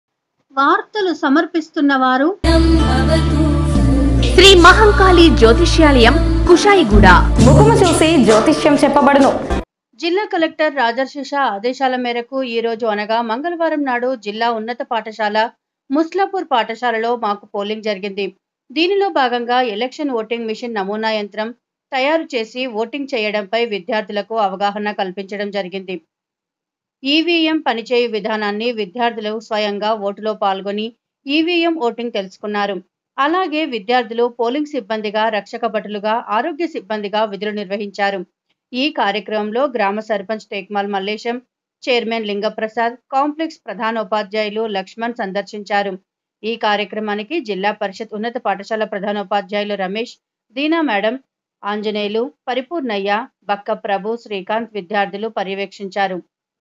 ఎలక్షన్ ఓటింగ్ మిషన్ నమూనా యంత్రం తారు చేసి ఓటింగ్ చేయడంపై విద్యార్థులకు అవగాహన
జిల్లా కలెక్టర్ రాజర్షఆదేశాల మేరకు ఈరోజు అనగా మంగళవారం నాడు జిల్లా జిల్లా ఉన్నత పాఠశాల ముస్లాపూర్ పాఠశాలలో మాకు పోలింగ్ జరిగింది, దీనిలో భాగంగా ఎలక్షన్ ఓటింగ్ మిషన్ నమూనా యంత్రం తారు చేసి ఓటింగ్ చేయడంపై విద్యార్థులకు అవగాహన కల్పించడం జరిగింది